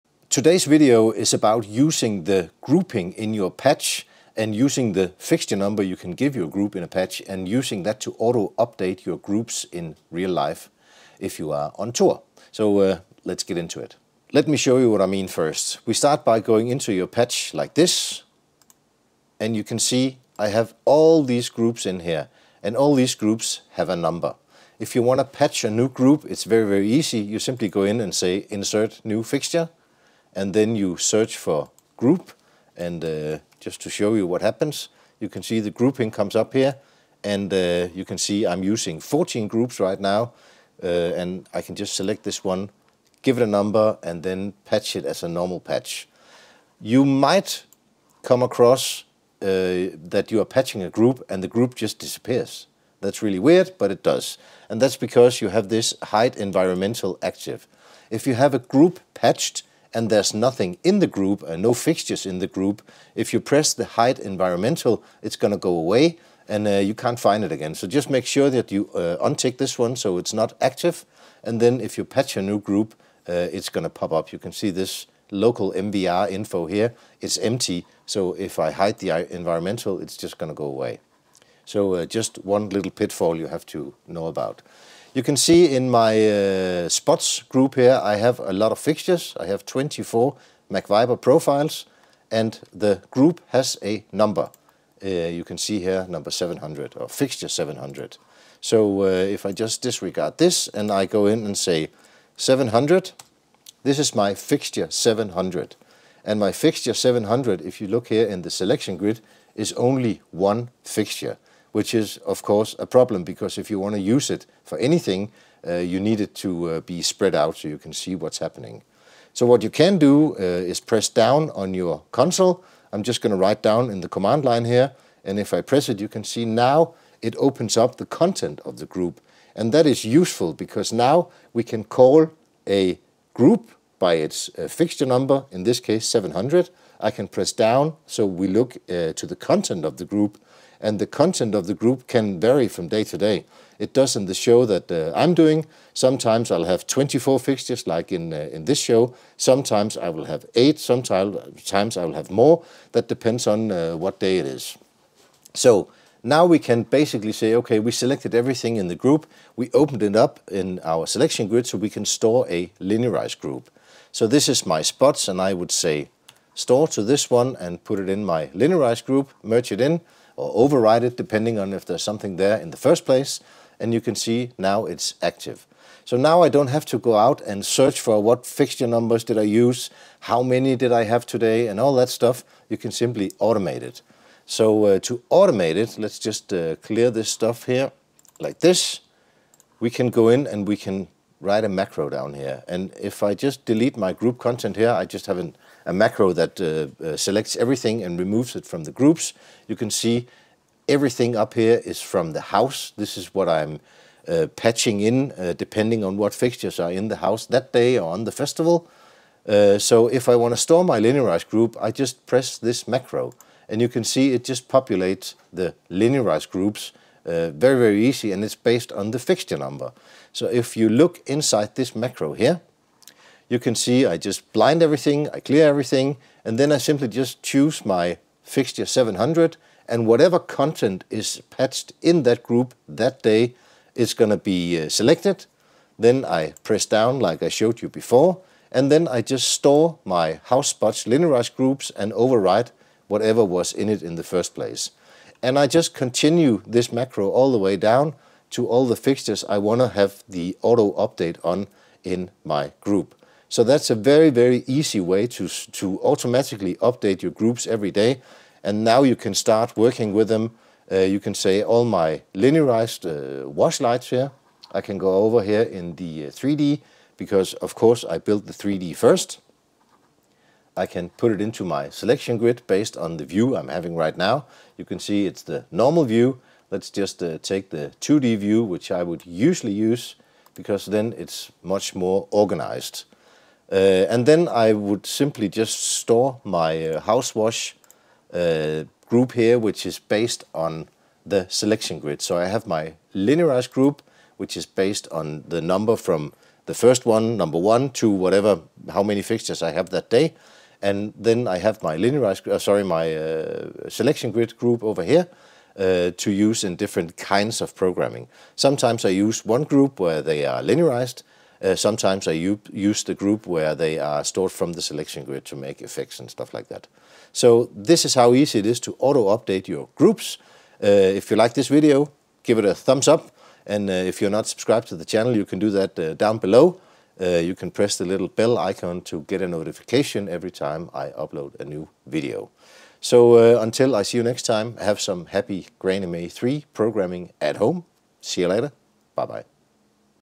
GrandMA3 Auto Update Groups
Welcome to another grandMA3 tutorial! 🚀 In this comprehensive guide, we'll explore the powerful functionality of grandMA3 and demonstrate the ease of Auto Update Groups using Macros.

Understanding how to leverage Fixture Grouping in the Patch is crucial for efficient lighting control. This video takes you through the process step by step, highlighting the significance of Auto Update Groups and showcasing the creation of Macros for seamless automation.

Learn setting up Auto Update Macros and witness a real-time demonstration of updating groups effortlessly. Along the way, we'll share valuable tips and tricks to optimize your workflow, ensuring you unlock the full potential of your grandMA3 console.

Whether you're a seasoned grandMA3 user or a beginner looking to enhance your lighting control skills, this tutorial provides valuable insights.